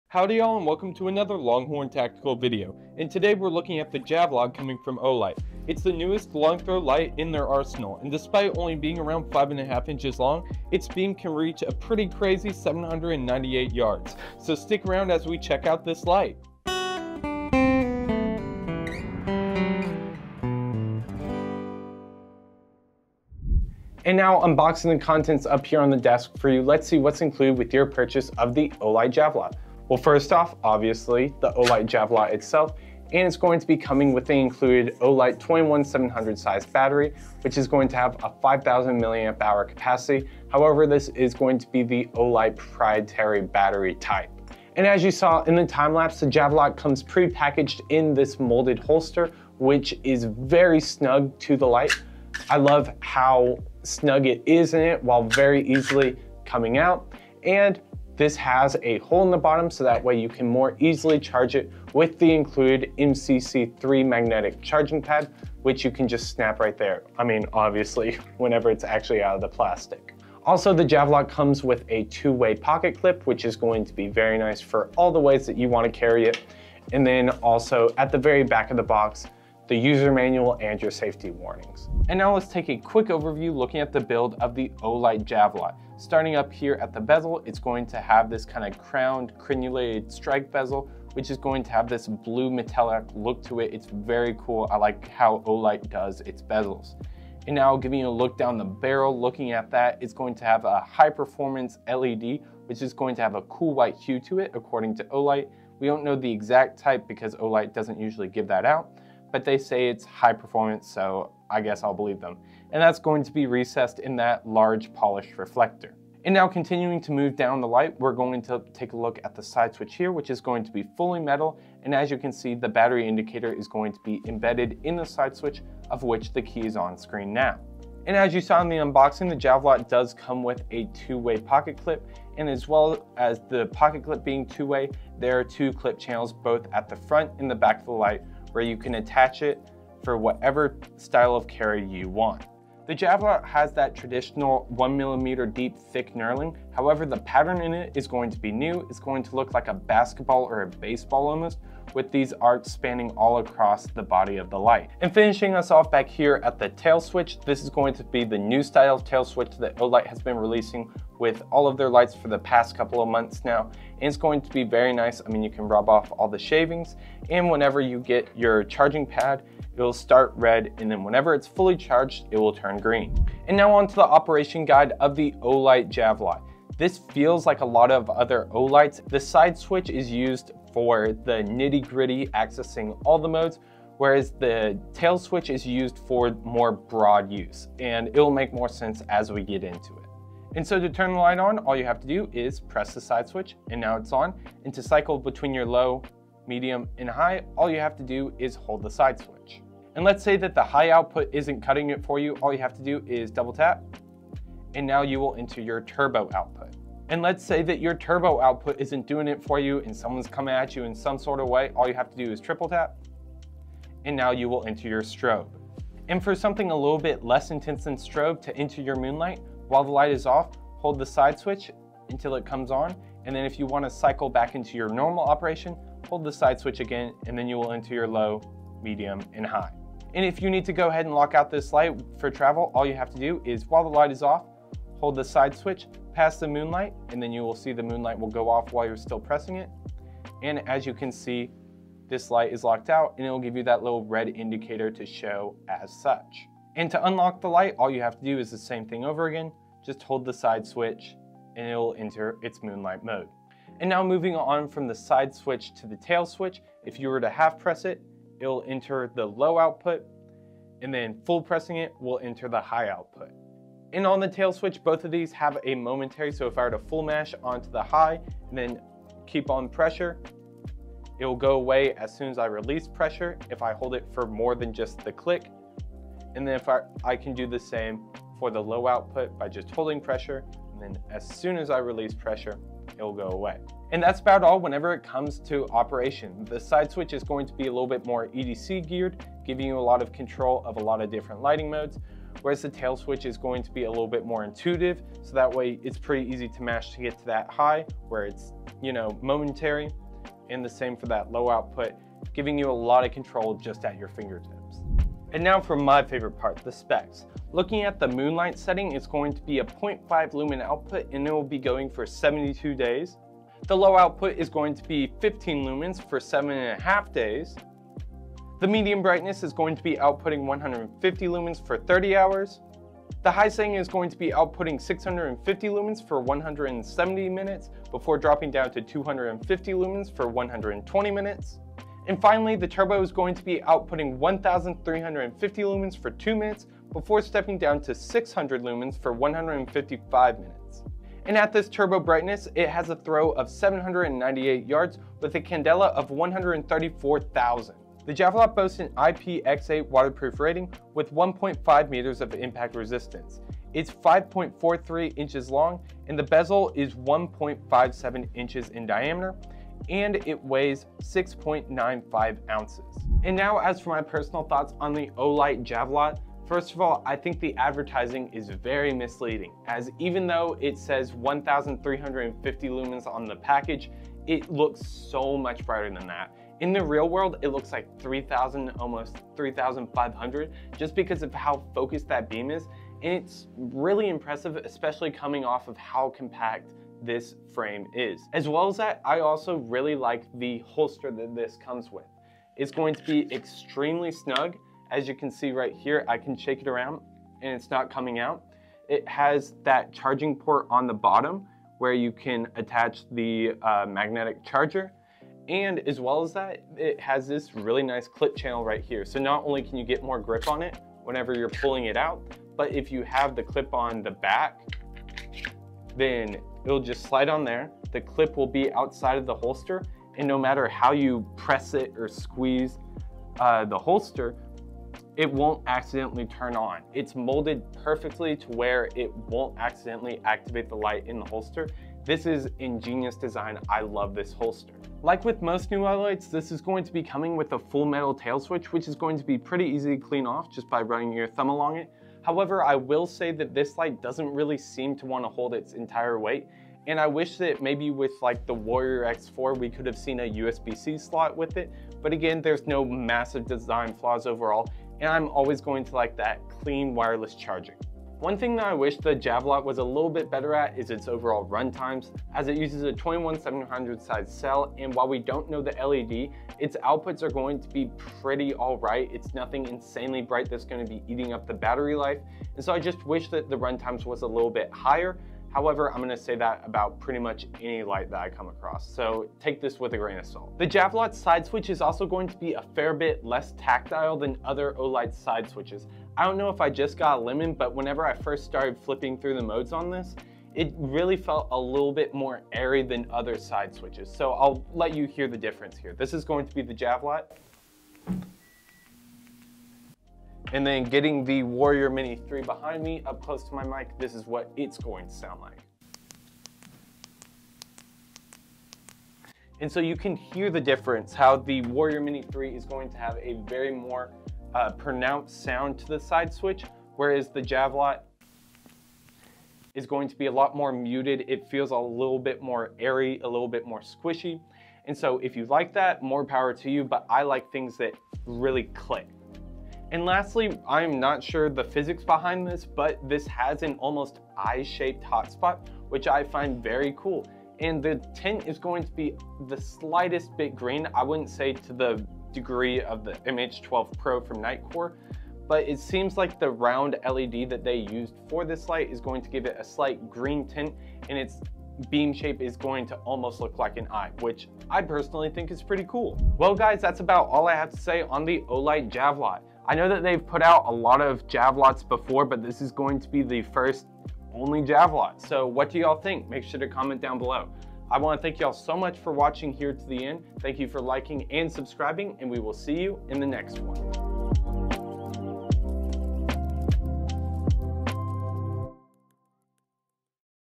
Olight Javelot = Long Range + Compact | 1350lm Long Throw Flashlight 🔦
Available at Longhorn Tactical

0:00 - Intro
0:24 - Unboxing
1:22 - Build Quality
2:53 - Operation Guide
5:07 - The Specs!
6:03 - Personal Thoughts
9:16 - Outro
Music used courtesy of Lofiru